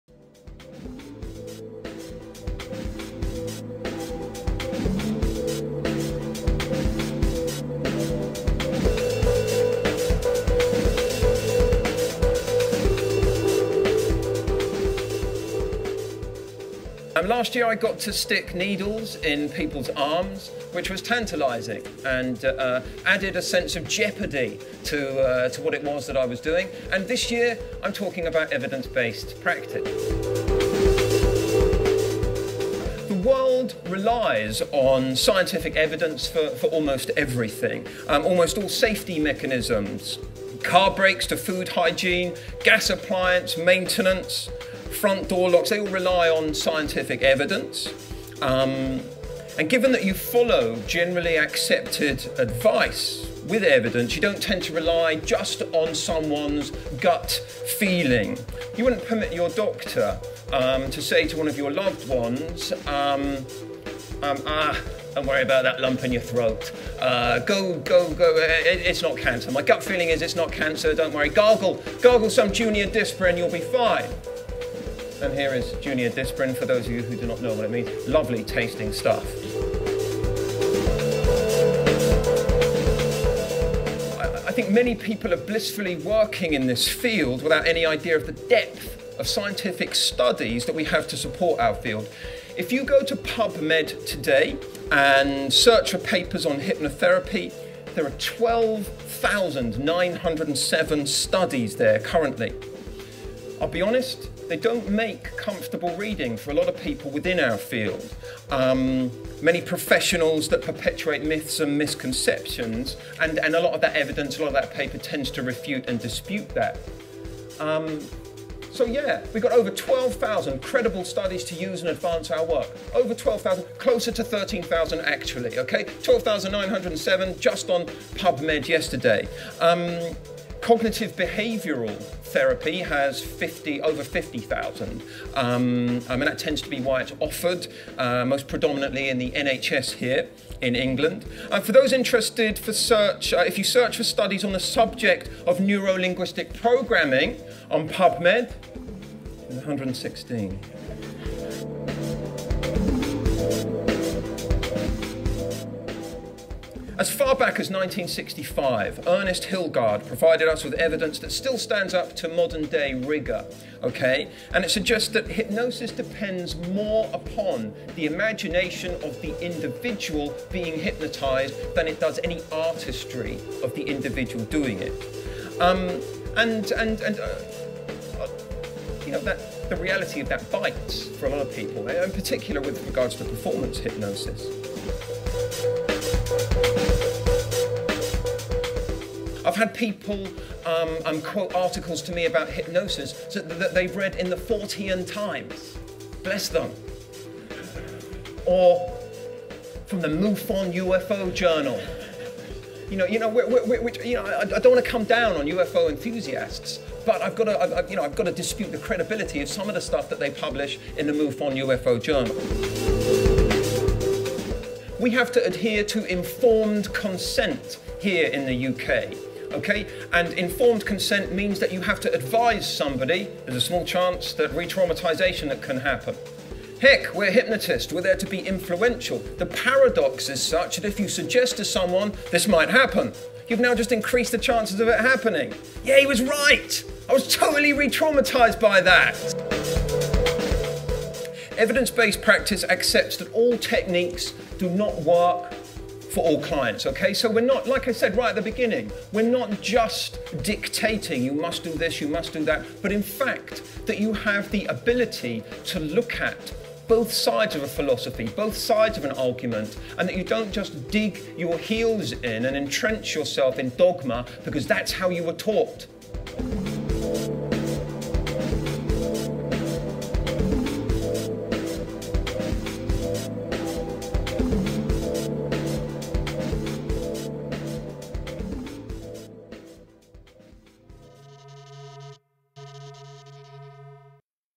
Adam Eason - Evidence Based Hypnosis - Change Phenomena 2013 Trailer
Leading hypnotherapist, trainer and author, Adam Eason, returns to discuss the application of scientific evidence to hypnotherapy, and why it is important in progressing the field.

This lecture was recorded at change | phenomena 2013, the hypnotism conference, in London, May 2013.  The full lecture is available as a digital download or as part of the DVD set